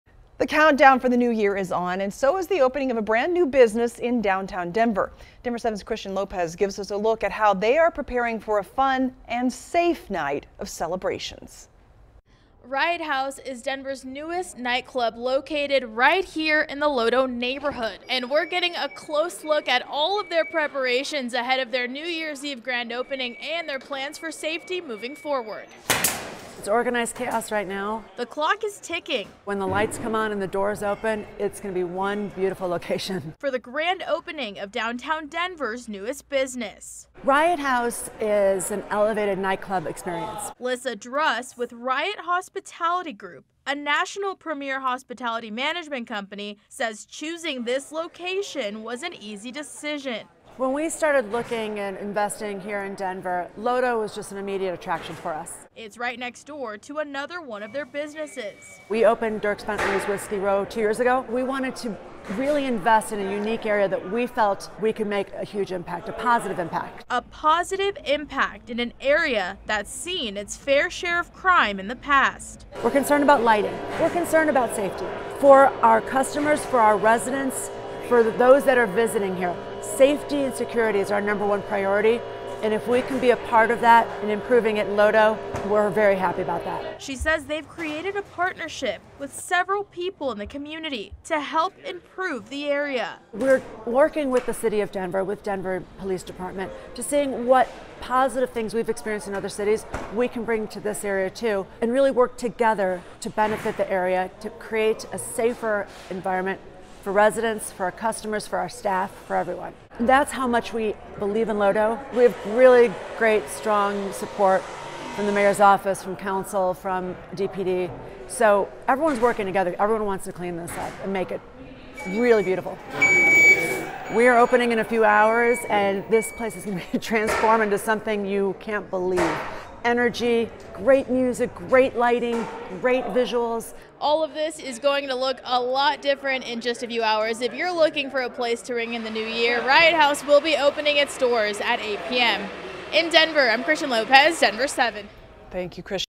New Denver nightclub Riot House celebrating grand opening on NYE
A brand new business in Denver will be celebrating its grand opening on New Year's Eve.